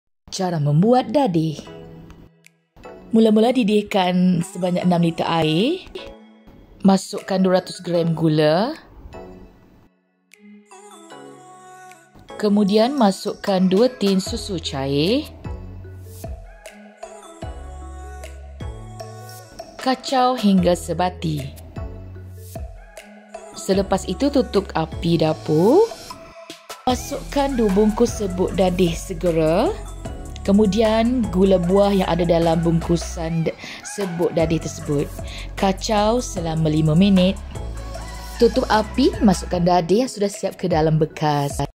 Cara Membuat Dadih Jagung
Credit
Musician: LiQWYD

Dadih yang dihasilkan ini menggunakan 2 bungkus dadih segera. Dadih ini boleh dapat 78 bekas dadih tinggi 4cm (4oz)